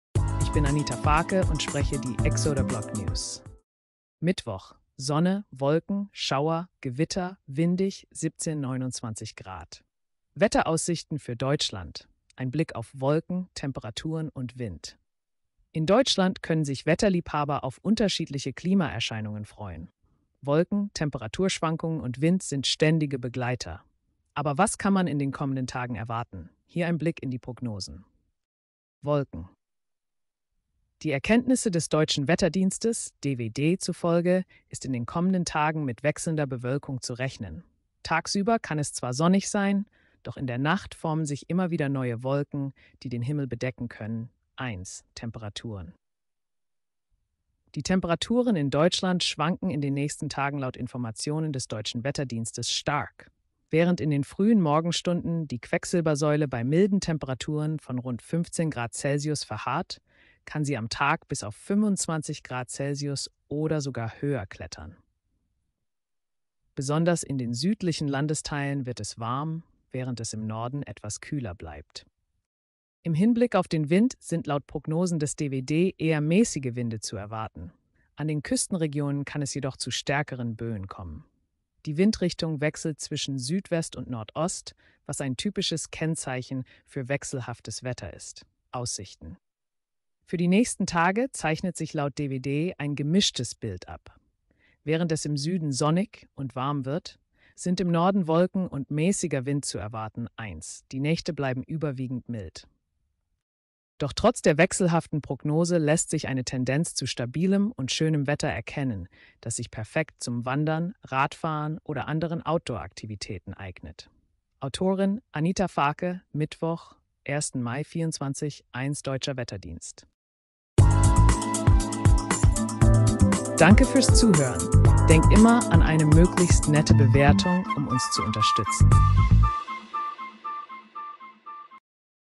Mittwoch: Sonne, Wolken, Schauer, Gewitter, windig 17-29 Grad (Podcast)
In den kommenden Tagen erwartet Deutschland wechselnde Bewoelkung, mit sonnigen Tagen und bewoelkten Naechten. Die Temperaturen schwanken stark, mit milden morgendlichen Temperaturen um 15 Grad Celsius, die im Laufe des Tages auf bis zu 25 Grad Celsius steigen koennen. Der Wind wird eher maessig sein, aber in Kuestengebieten koennen staerkere Boeen auftreten. Im Sueden wird es sonnig und warm, waehrend im Norden Wolken und maessiger Wind dominieren. Trotz der wechselhaften Prognose gibt es eine Tendenz zu stabilem und schoenem Wetter, das sich ideal fuer Outdoor-Aktivitaeten eignet. AI-generated content